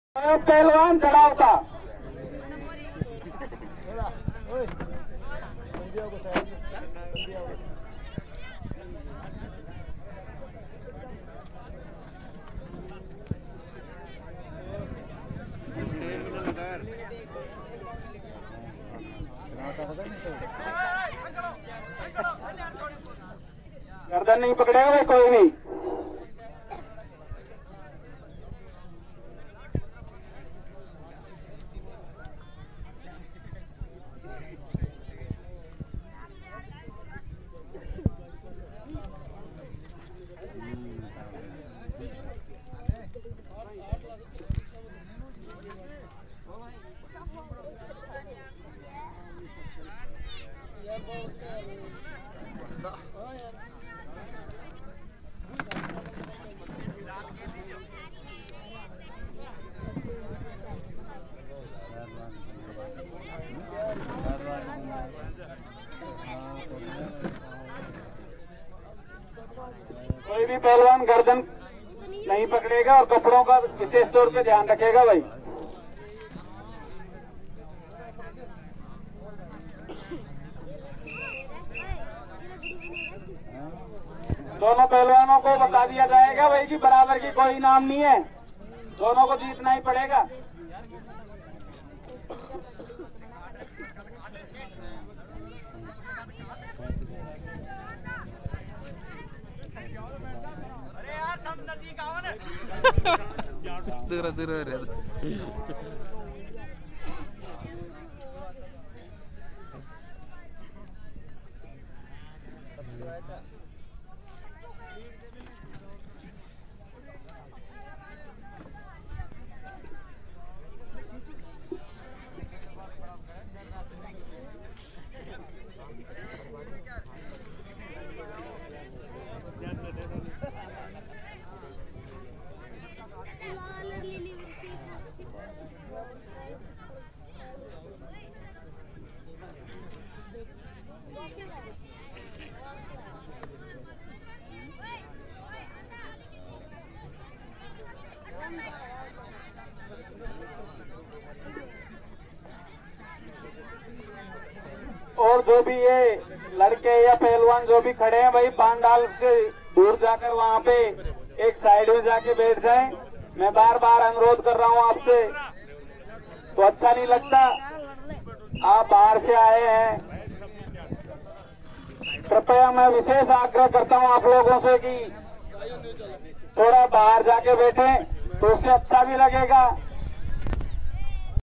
सवाई माधोपुर कुस्ती दंगल !! जड़ावता का पहलवान vs खाट का पहलवान !! शानदार कुस्ती वीडियो part 4
LMG STAR

खाट का पहलवान vs जड़ावता का पहलवान

LMG STAR

यह कुस्ती दंगल हर साल करवाया जाता है
यहा पर स्थानीय पहलवानो के अलावा दुर-दराज के पहलवान कुस्ती लडने को आते है,
सालो से चले आ रहे ईस दंगल को गांव वालो की मदद से हर वर्ष की तरह ईस बार भी करवाया गया है ।
ओर अगली बार भी करवाया जायेगा
अगर आप भी यहा पर आना चाहते है तो आ सकते है

--कब ओर कैसे आये यहां पर--

हर वर्ष 19 फरवरी को आ सकते है
यह Date हमेशा ये ही रहेगी

स्थान--
गावं- जडावता
तह./जिला. सवाई माधोपुर
राजस्थान
322027

आने का रास्ता-
बस द्वारा

- दौसा,गंगापुर सिटी की ओर से आने वालो को दुब्बी बनास ऊतरना है,ओर जडावता का रास्ता मील जायेगा !

अगर आप सवाई माधोपुर होते हुये आते है तो आपको पढाना मोड पर ऊतरना है, पास ही जडावता है

रेलगाडी द्वारा-
सवाई माधोपुर स्टेशन पर ऊतरना है
ओर बस द्वारा आप पढाना मोड ऊतरना है
दुब्बी बनास भी ऊतर सकते है (यह ज्यादा ठीक रहेगा)

अगर फिर भी कोई दिक्कत आये तो आप हमे Comment सकते है

~Note-- याद रहे हमैशा 19 फरवरी को ही यह कुस्ती दंगल होता है~